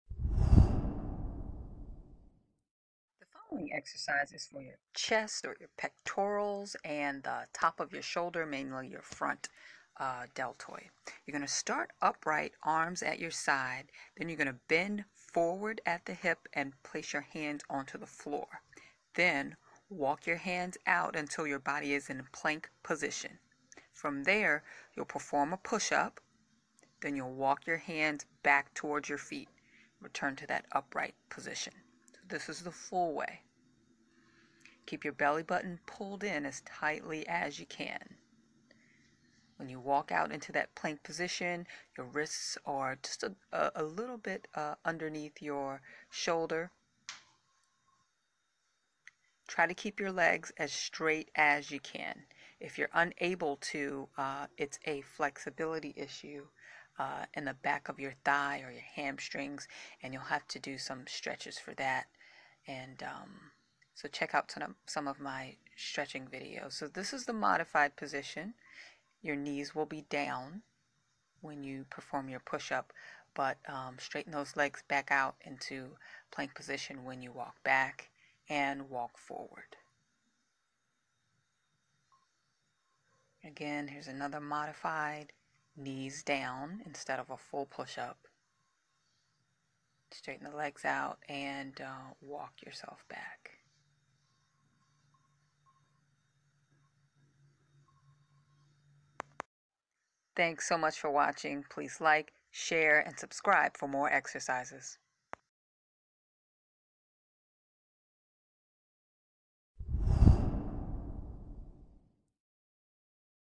In Home Strength Exercise (Chest & Shoulders): Walk Out Push Up
Equipment & Substitute(s):  n/a

Target Muscle(s): Chest (Pectorals), Front Shoulder (Anterior Deltoid)

Goal(s): Strength

To Start: Begin upright with your arms at your sides. Pull your belly button into your spine.

Full Version: Walk your hands out until your body is in plank position with your hands under your shoulders. Perform a push up, and walk your hands back to your feet. Return to the upright start. Repeat.

Modified Version: Same as above, except bend your knees, placing your knees down onto the floor for the push up portion.

Fitness Tip(s): Breath naturally throughout the movements. Keep your belly button pulled into your spine throughout.

Thank you for your support and positivity everyone! It will help to keep these videos super low cost for me to create.

Octavia Henderson (i.e. Oh) is a certified personal trainer by ACSM & registered kinesiotherapist by AKTA.